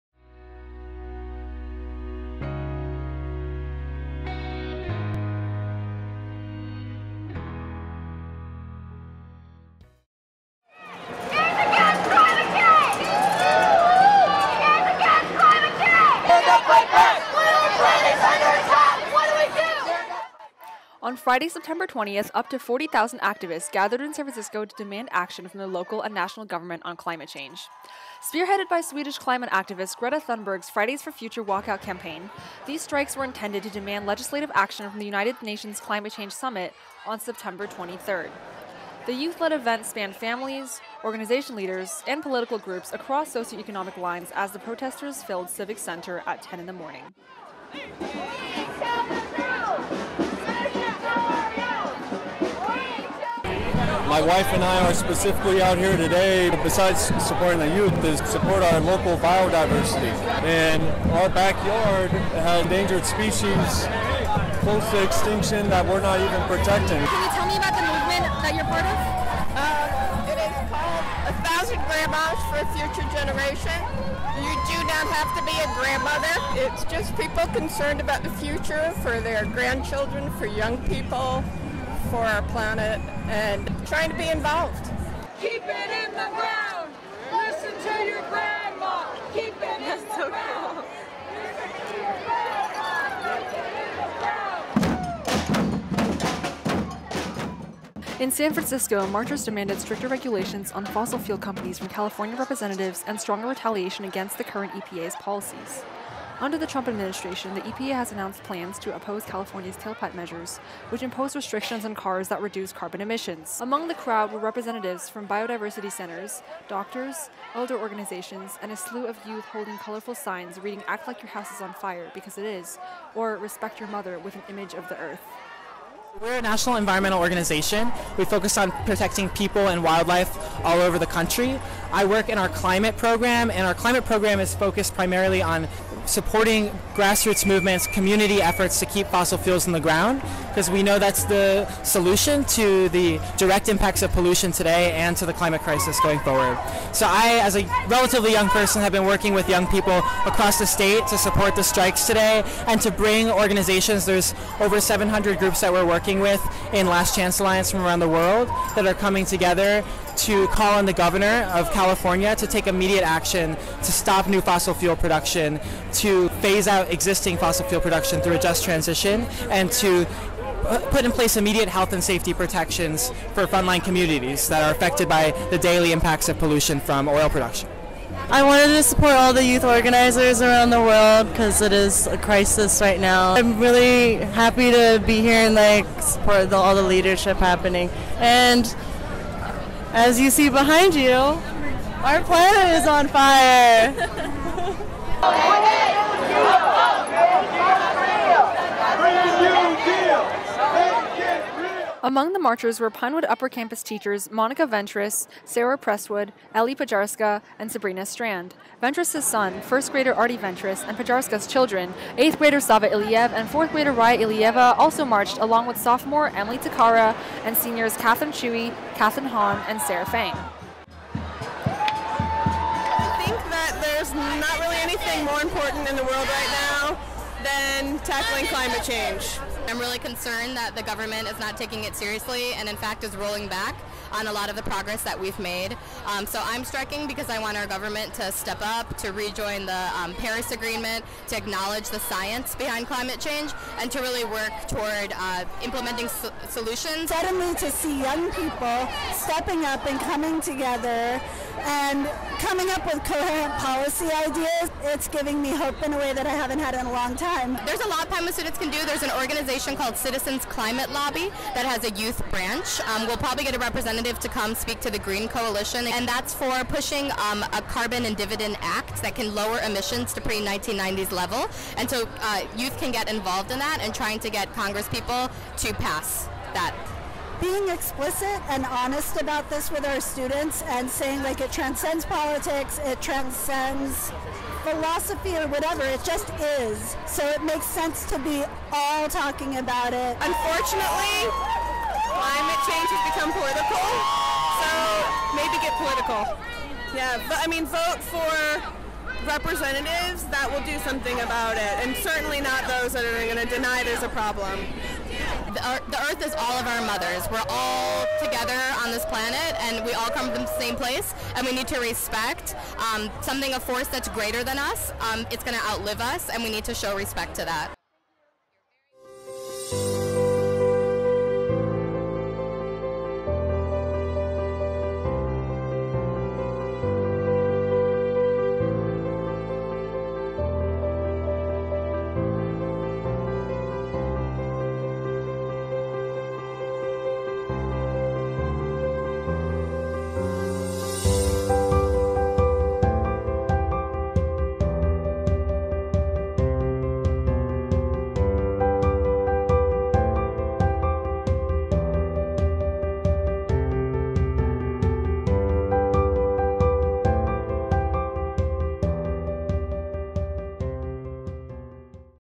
The Fight Against Climate Change | The Digital Perennial
Pinewood faculty and students join the FridaysforFuture Youth Climate Strike in San Francisco.

Interviewer: Sarah Feng
Photography By: Katherine Chui
Video Produced By: Katherine Han

Check out the full article "Pinewood Protestors March For Climate" in the September 2019 issue of The Perennial.